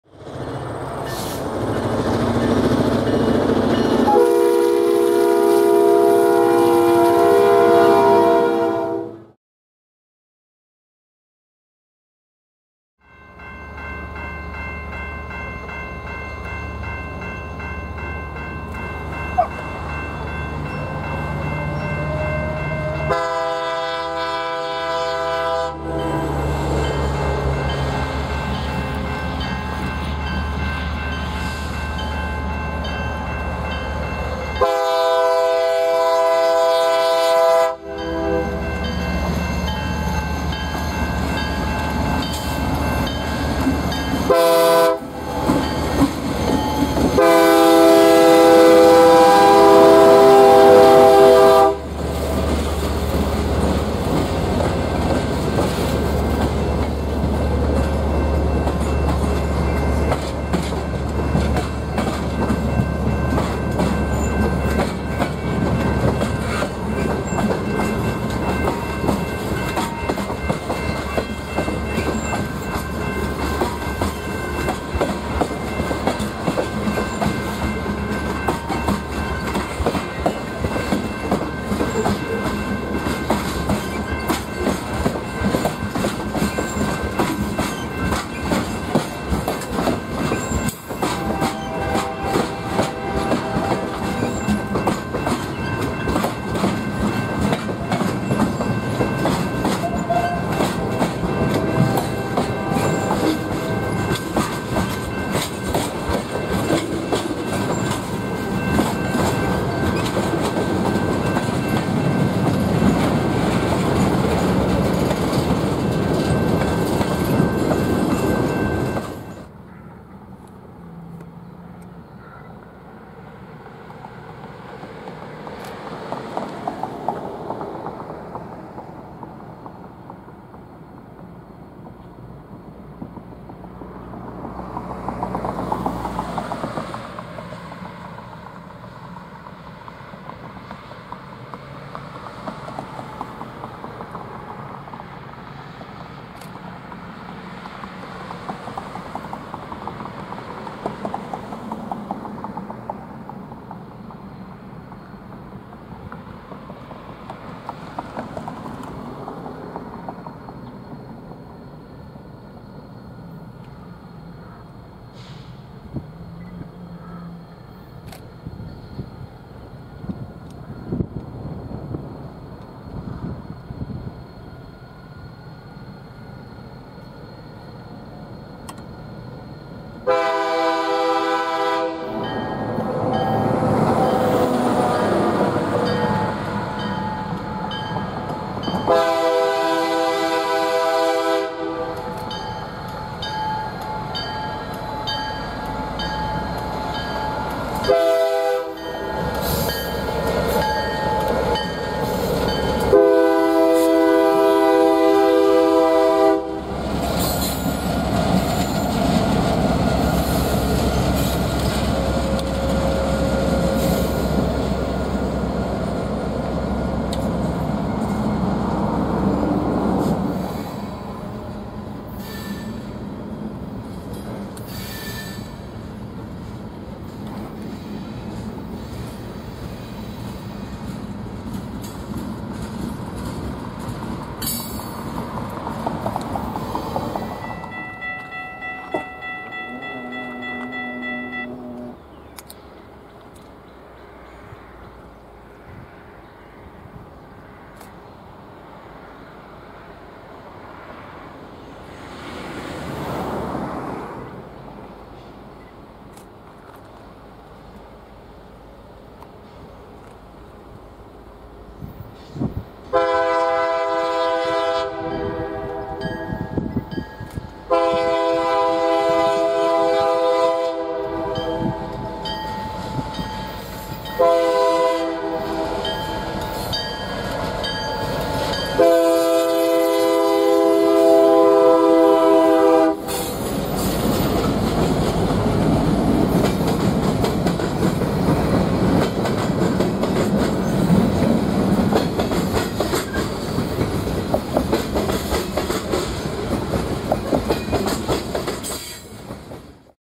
CP 6024 (SD40-2) leads K70 from Nahant Yard to Montpelier, IA - Tuesday, June 20, 2023
CP 6024, an SD40-2, is seen at three locations after departing Nahant Yard. It and CP 5034 are in charge of the afternoon K70, a Nahant local that runs down to the HARSCO Steel Mill in Muscatine County.